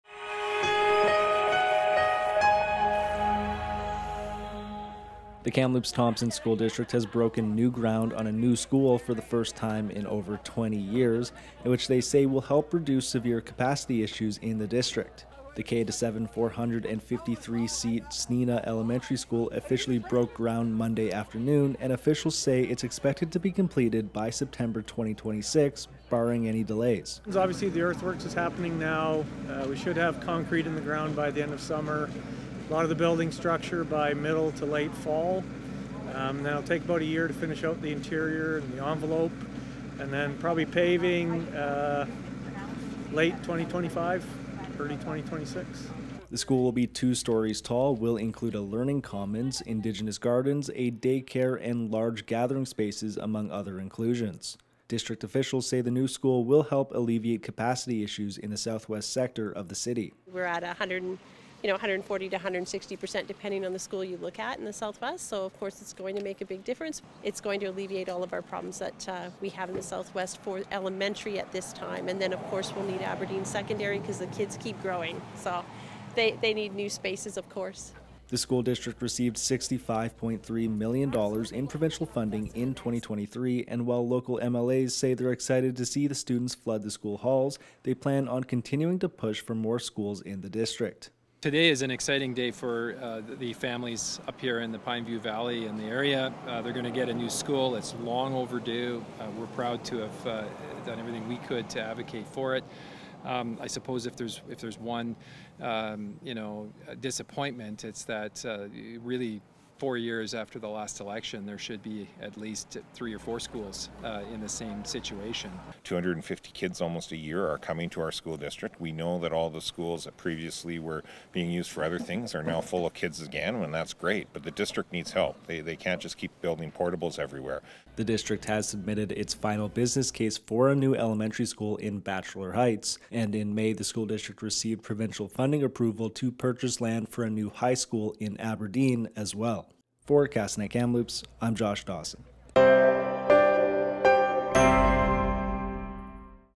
School District breaks new ground
The Kamloops-Thompson school district has broken new ground on a new school for the first time in over 20 years with the aim of reducing severe capacity issues in the district.